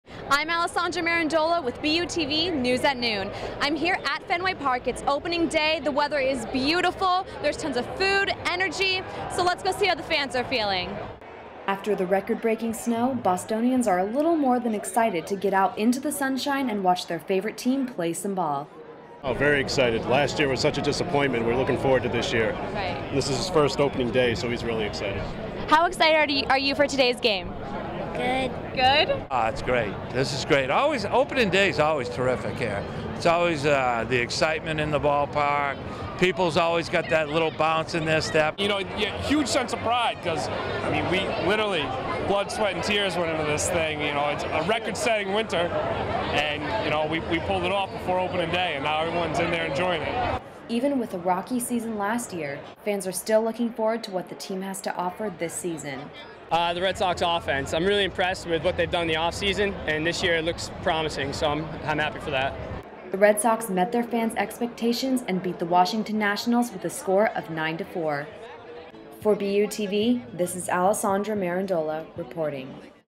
Fans Flock to Red Sox Home Opener
The Red Sox season opener at Fenway Park sure proved to be an exciting one. People from all over Massachusetts came out to enjoy the sunshine and their favorite baseball team win their first game of the season. BUTV10's Alessandra Marandola reports from Fenway.